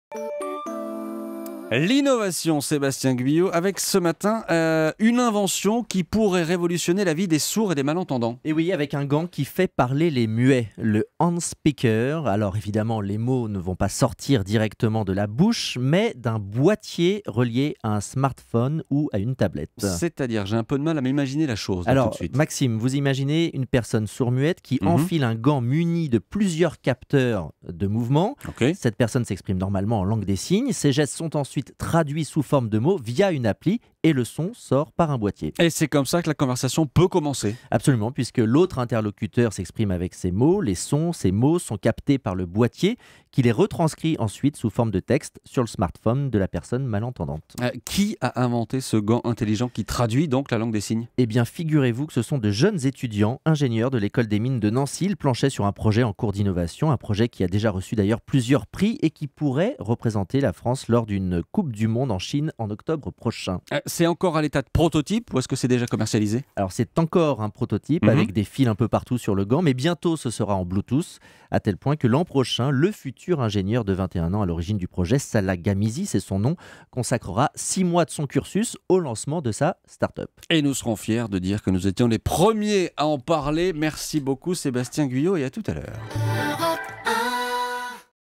Le gant qui fait parler les muets
Sébastien Guyot revient sur ce gant intelligent qui traduit la langue des signes.